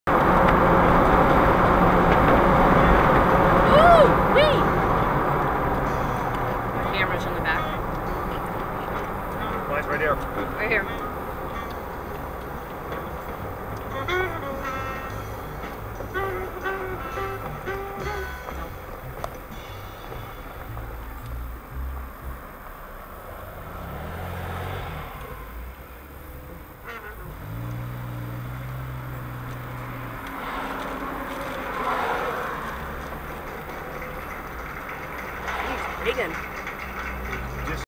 Moose
This big bastard crossed in front of us (he's there, look closely) when on a get-away this summer.